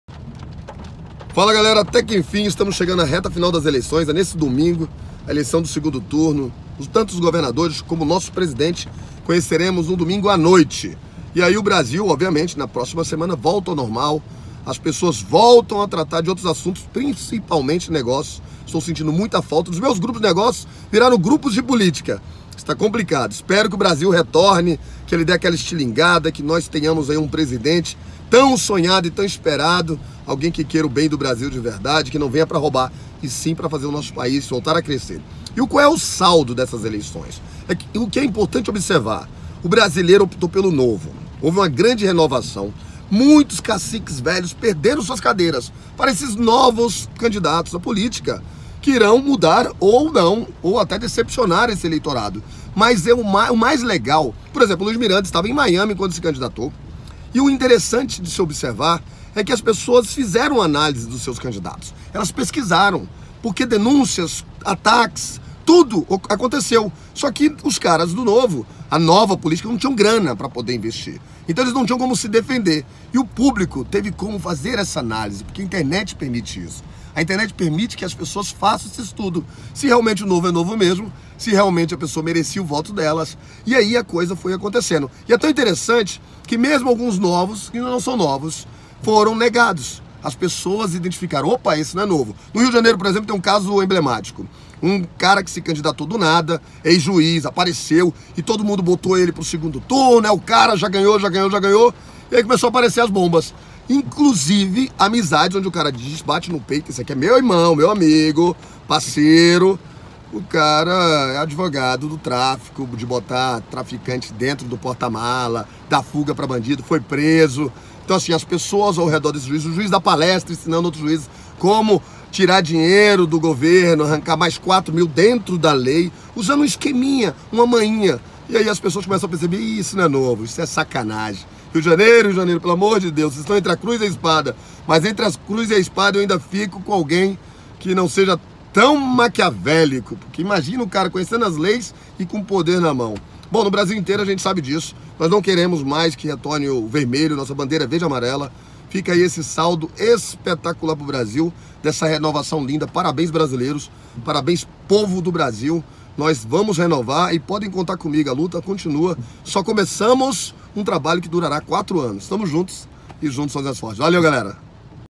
Foi incrível... a eleição das renovações
Foi incrível... a eleição das renovações! Semana que vem tudo volta ao normal e com grandes expectativas de um Brasil melhor. No Rio de Janeiro a renovação é assustadora, porém no resto do Brasil as eleições estão bem definidas e Brasília o Ibaneis está com a maior vantagem do Brasil. Eita final de semana que será longo!